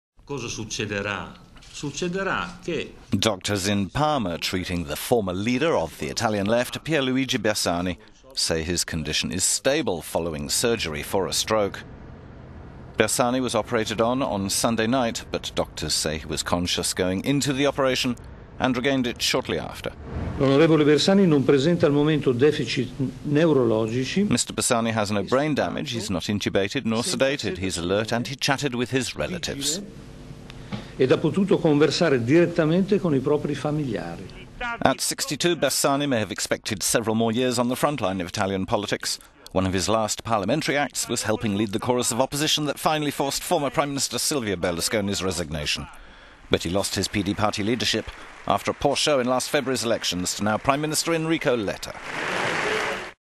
Italian politician Pier Luigi Bersani is stable after stroke
Doctors treating Pier Luigi Bersani, former leader of the Italian centre-left, say his condition is...

Doctors treating Pier Luigi Bersani, former leader of the Italian centre-left, say his condition is stable after surgery for a brain haemorrhage.

The unexepcted illness of Bersani, who underwent surgery on Sunday night, prompted an outpouring of sympathy and support, including from Prime Minister Enrico Letta and rivals Silvio Berlusconi and Beppe Grillo.

"Mr. Bersani has no brain damage, he is not intubated, nor sedated. He's alert, and he chatted with his relatives," said Leonida Grisendi, general director of Parma's Maggiore Hospital.

But Bersani's surgeon, Ermanno Giombelli, was cautious in his comments during a press conference broadcast from a hospital in the northern city of Parma, saying complications can follow such surgery several days later.